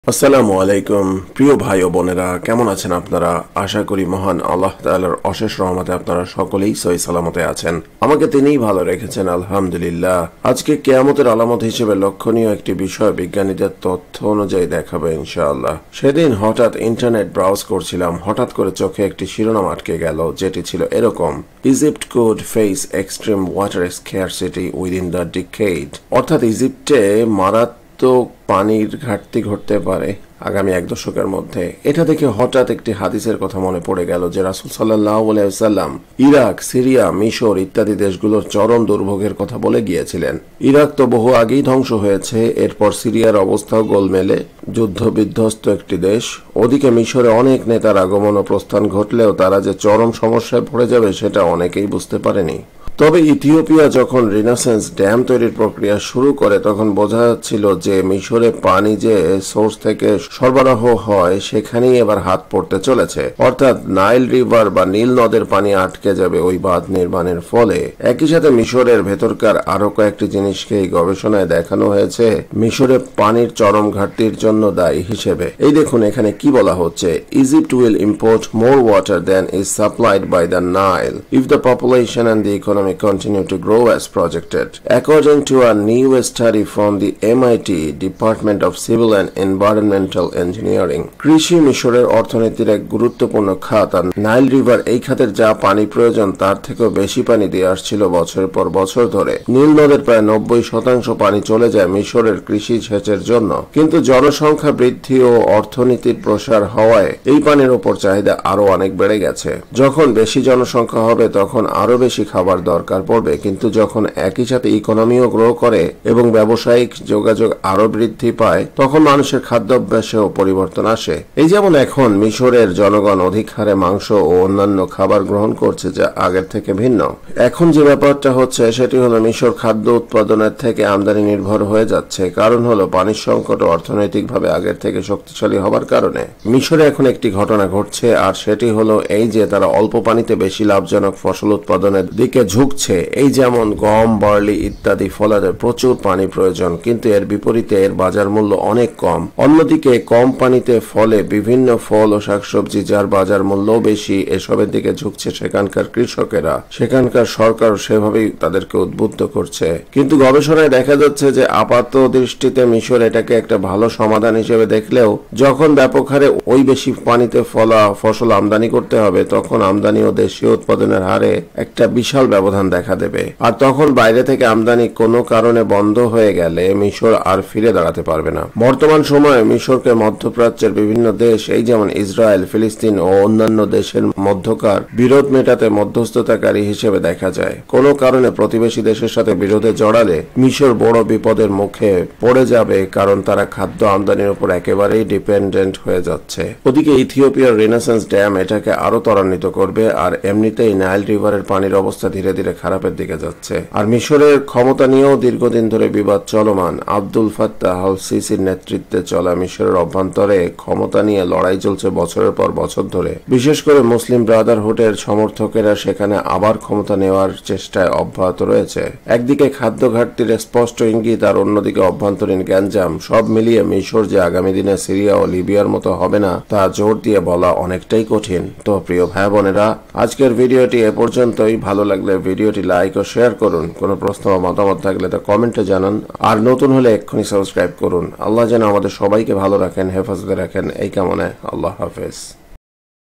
শেষ সময়ে মিশরে চরম খাদ্য ঘাটতি দেখা দেবে আর MIT সূত্র বলছে আগামী দশ বছরের মধ্যে বিশাল বড় একটা...
ইরাক তো বহু আগেই ধ্বংস হয়েছে, এরপর সিরিয়ার অবস্থাও গোলমেলে। ওদিকে মিশরে অনেক নেতার আগমন ও প্রস্থান ঘটলেও তারা যে চরম সমস্যায় পড়ে যাবে সেটা অনেকেই বুঝতে পারেনি।

তবে ইথিওপিয়া যখন রিনসেন্স ড্যাম তৈরির প্রক্রিয়া শুরু করে তখন বোঝা যাচ্ছিল যে মিশরে পানি যে সোরস থেকে সরবরাহ হয় সেখানেই এবার হাত পড়তে চলেছে অর্থাৎ নাইল রিভার বা নীল নদের পানি আটকে যাবে ওই বাধ নির্মাণের ফলে।

একই সাথে মিশরের ভেতরকার আরো কয়েকটি জিনিসকে এই গবেষণায় দেখানো হয়েছে মিশরে পানির চরম ঘাটতির জন্য দায়ী হিসেবে।

কৃষি মিশরের অর্থনীতির এক গুরুত্বপূর্ণ খাত আর নাইল রিভার এই খাতের যা পানি প্রয়োজন তার থেকেও বেশি পানি দিয়ে আসছিল বছরের পর বছর ধরে।
নীল নদের প্রায় ৯০% পানি চলে যায় মিশরের কৃষি সেচের জন্য, কিন্তু জনসংখ্যা বৃদ্ধি ও অর্থনীতির প্রসার হওয়ায় এই পানির ওপর চাহিদা আরো অনেক বেড়ে গেছে।

যখন বেশি জনসংখ্যা হবে তখন আরো বেশি খাবার দরকার পড়বে। কিন্তু যখন একইসাথে ইকোনোমিও গ্রো করে এবং ব্যাবসায়িক যোগাযোগ আরো বৃদ্ধি পায় তখন মানুষের খাদ্যভ্যাসেও পরিবর্তন আসে।

এই যেমন এখন মিশরের জনগন অধিকহারে মাংস ও অন্যান্য খাবার গ্রহন করছে যা আগের থেকে ভিন্ন।

এখন যে ব্যাপারটা হচ্ছে সেটি হল মিশর খাদ্য উৎপাদনের থেকে আমদানি নির্ভর হয়ে যাচ্ছে কারণ হল পানির সংকট ও অর্থনৈতিকভাবে আগের থেকে শক্তিশালী হবার কারণে।

মিশরে এখন একটা ঘটনা ঘটবে আর সেটি হল এই যে তারা অল্প পানিতে বেশি লাভজনক ফসল উৎপাদনের দিকে ঝুকবে এই যেমন গম বার্লি ইত্যাদি ফলাতে প্রচুর পানি প্রয়োজন কিন্তু এর বিপরীতে এর বাজার মূল্য অনেক কম। অন্যদিকে কম পানিতে ফলে বিভিন্ন ফল ও শাকসবজি যার বাজার মূল্যও বেশি এসবের দিকে ঝুকছে সেখানকার কৃষকেরা। সেখানকার সরকারও সেভাবেই উদ্বুদ্ধ করছে।

কিন্তু গবেষণায় দেখা যাচ্ছে যে আপাত দৃষ্টিতে মিশর এটাকে একটা ভাল সমাধান হিসেবে দেখলেও যখন ব্যাপক হারে ওই বেশি পানিতে ফলা ফসল আমদানি করতে হবে তখন আমদানি ও দেশীয় উৎপাদনের হারে একটা বিশাল ব্যবধান দেখা দেবে।

আর তখন বাইরে থেকে আমদানি কোন কারণে বন্ধ হয়ে গেলে মিশর আর ফিরে দাঁড়াতে পারবে না।

বর্তমান সময়ে মিশরকে মধ্যপ্রাচ্যের বিভিন্ন দেশ এই যেমন ইসরাইল, ফিলিস্তিন, ও অন্যান্য দেশের মধ্যেকার বিরোধ মেটাতে মধ্যস্থতাকারী হিসেবে দেখা যায়। কোন কারণে প্রতিবেশি দেশের সাথে বিরোধে জড়ালে মিশর বড় বিপদের পড়ে যাবে কারণ তারা খাদ্য আমদানির ওপর একেবারেই ডিপেন্ডেন্ট হয়ে যাচ্ছে।

ওদিকে ইথিওপিয়ার রিনসেন্স ড্যাম এটাকে আরো তরান্বিত করবে আর এমনিতেই নাইল রিভারের পানির অবস্থা ধীরে ধীরে খারাপের দিকে যাচ্ছে।

আর মিশরের ক্ষমতা নিয়েও দীর্ঘদিন ধরে বিবাদ চলমান। আব্দুল ফাত্তাহ আল সিসির নেতৃতে চলা মিশরের অভ্যান্তরে ক্ষমতা নিয়ে লড়াই চলছে বছরে পর বছর ধরে। বিশেষ করে মুসলিম ব্রাদারহুডের সমর্থকেরা সেখানে আবার ক্ষমতা নেওয়ার চেষ্টায় অভ্যাহত রয়েছে।

একদিকে খাদ্যঘাটতির স্পষ্ট ইঙ্গিত আর অন্যদিকে অভ্যান্তরীন গ্যাঞ্জাম সব মিলিয়ে মিশর যে আগামী দিনে সিরিয়া ও লিবিয়ার মত হবে না তা জোর দিয়ে বলা অনেকটাই কঠিন।